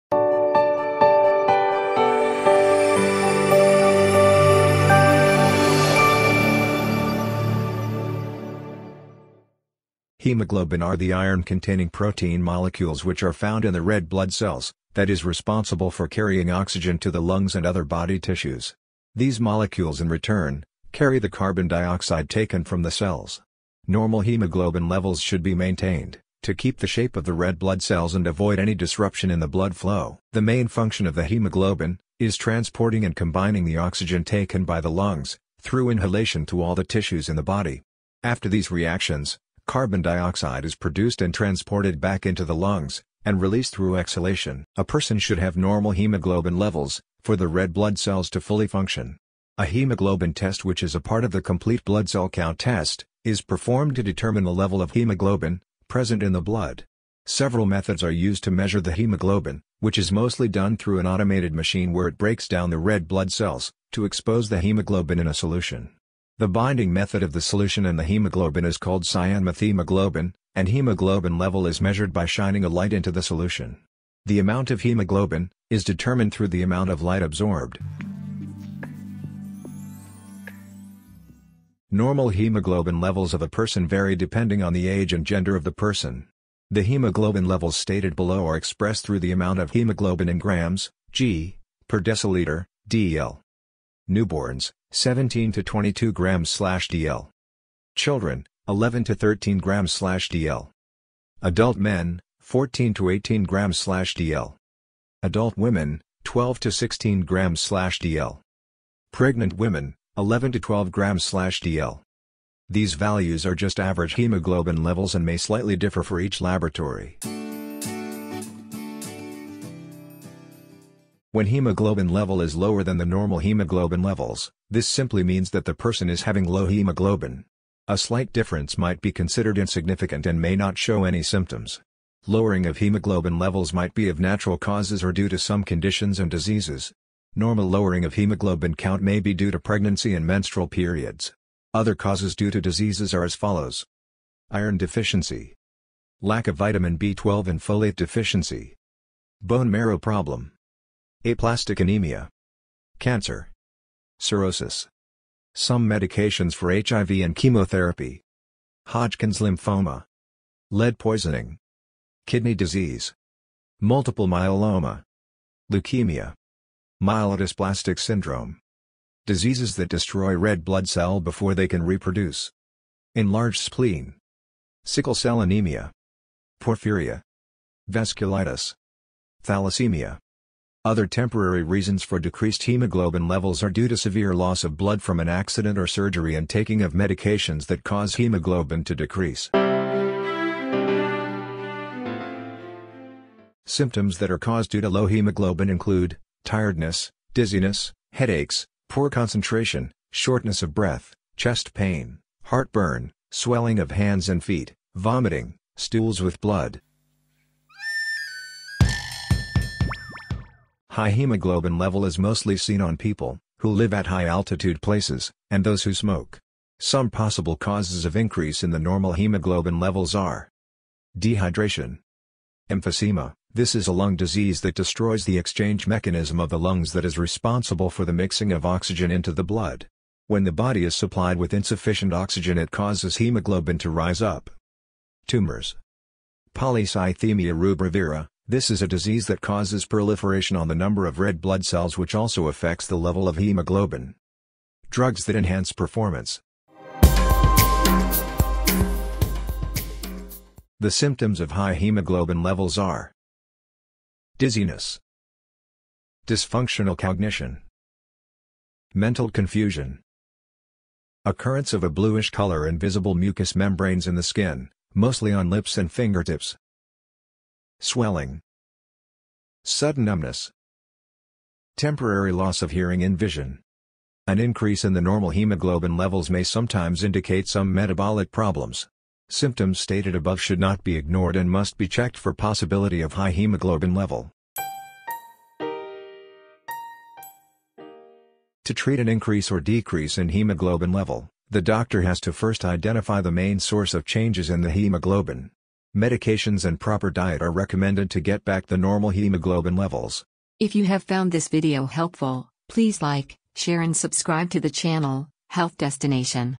Hemoglobin – Low and High Levels, Normal Range
Explanation of hemoglobin levels, normal range, causes for low and high levels and corresponding symptoms.
Easy Blood Typing kit for testing at home